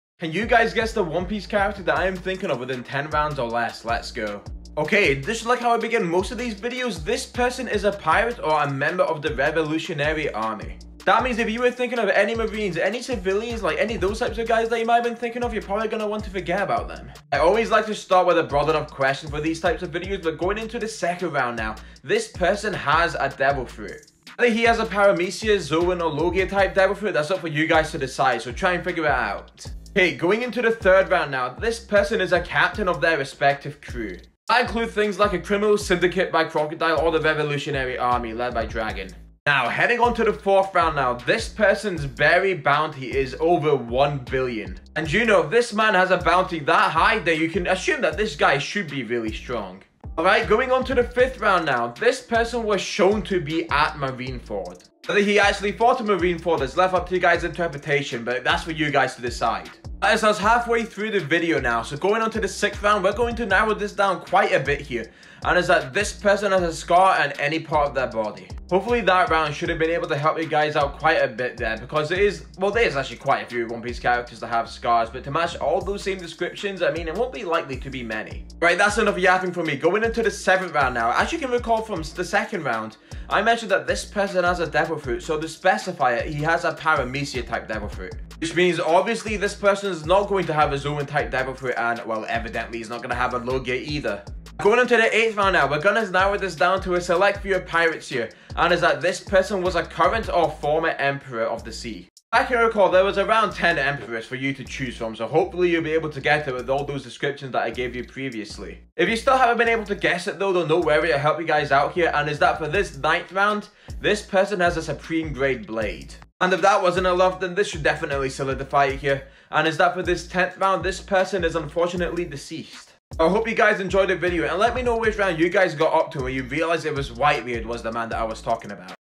GUESS THE ONE PIECE CHARACTER IN 10 ROUNDS OR LESS👀
Try to guess the one piece character that I am thinking of within 10 clues!